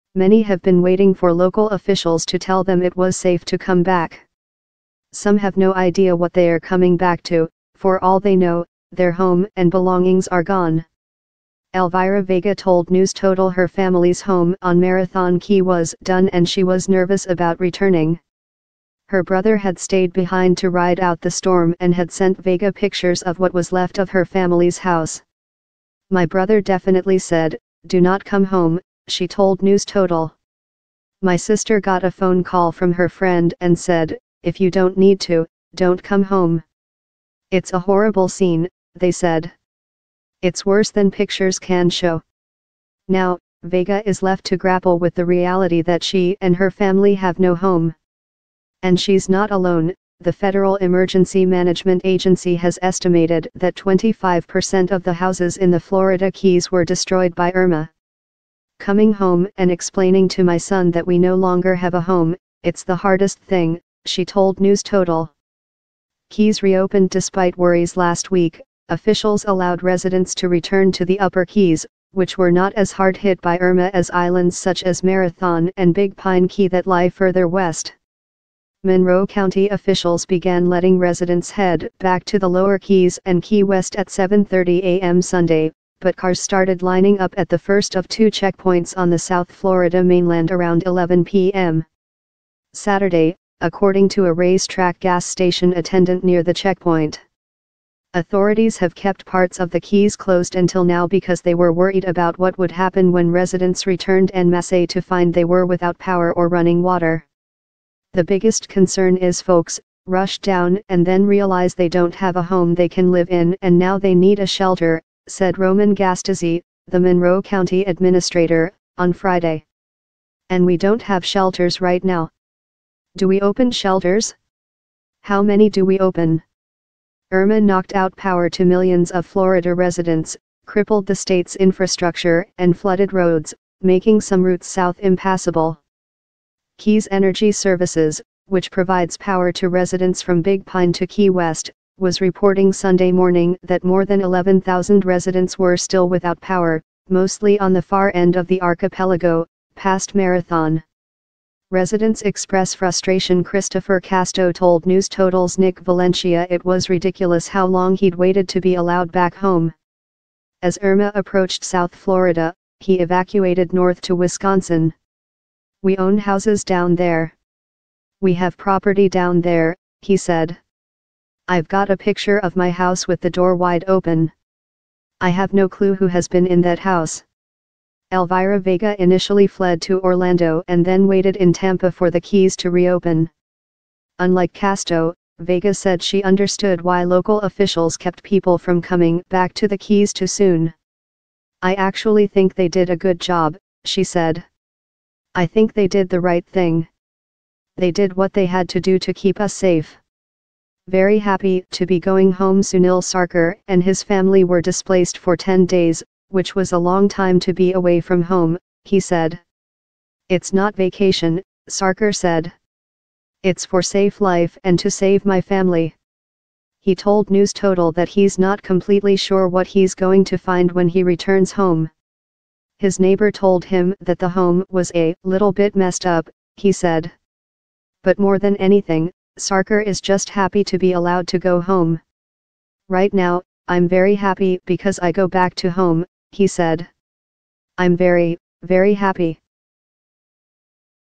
Irmas Aftermath: Residents Finally Returning To Lower Florida Keys
Many have been waiting for local officials to tell them it was safe to come back.